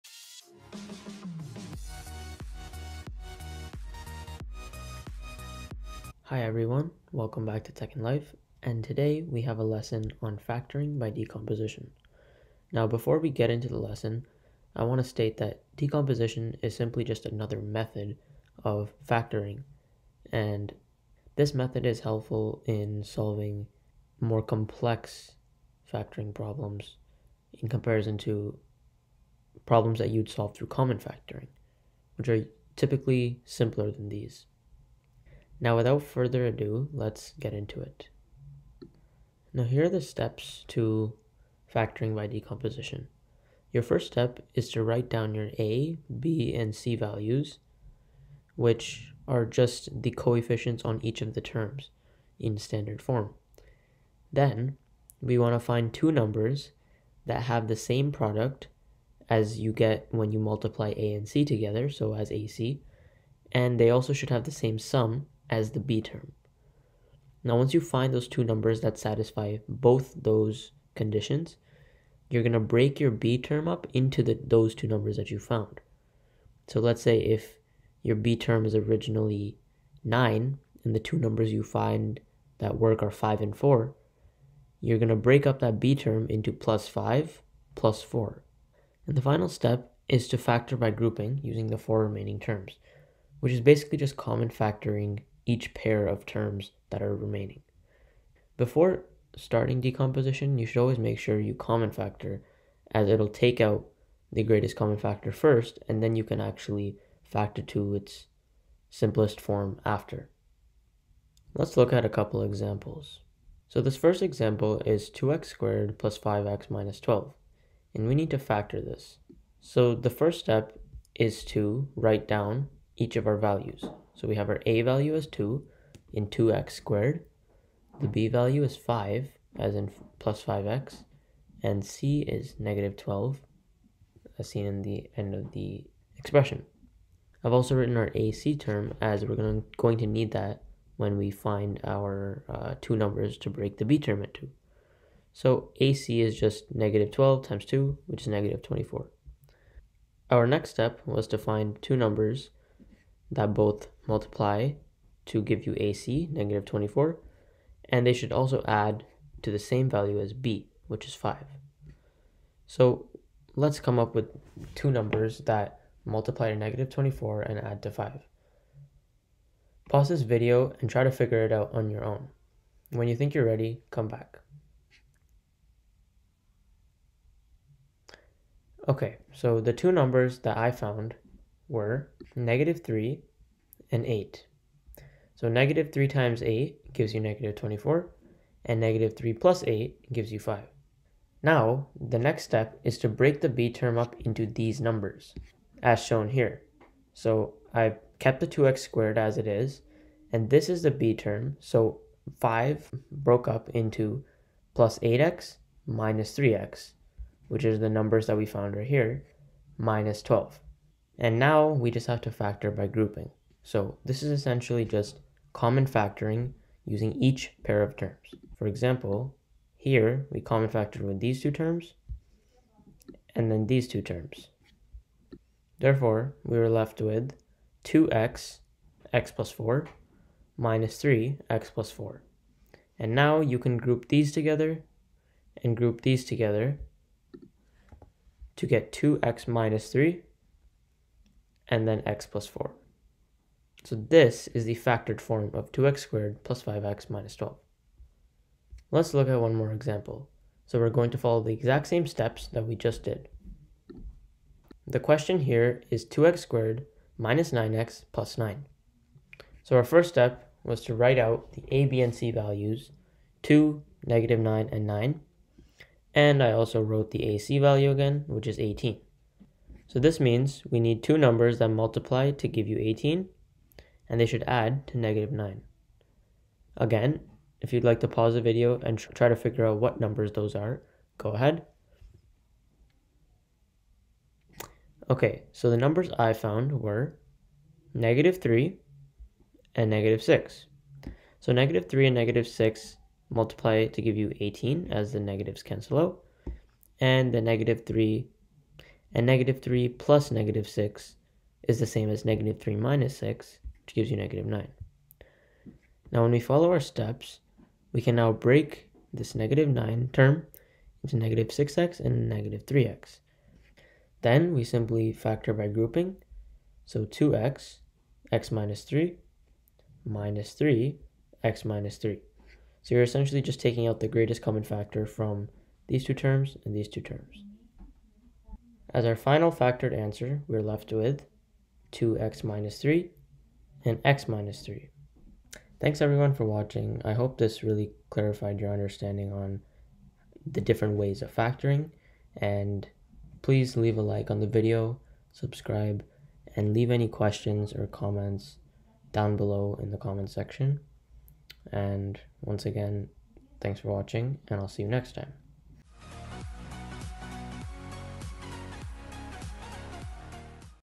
Factoring by Decomposition | Mathematics | TekkinLife
This video is a tutorial on how to common factor!

fun, grade 10, math, easy, simple, learn, linear, equations, factors, parabola, quadratics, decomposition, multiply, learn, zeros